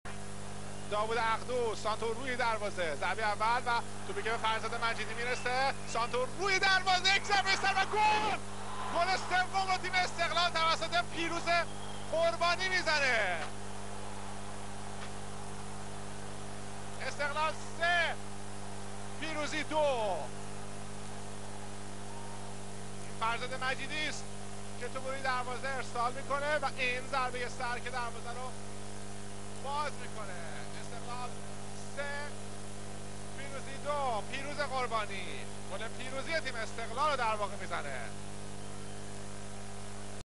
Gool 32 (83_84) ** Week 21 ** Pirooz Ghorbani ** Esteghlal _ Perspolis Tehran
صد و چهل و هشتمین گل استقلال در ادوار ليگ برتر
فصل 1383_1384
هفته بیست و یكم -- آزادي تهران
استقلال تهران -- پرسپولیس تهران
پیروز قرباني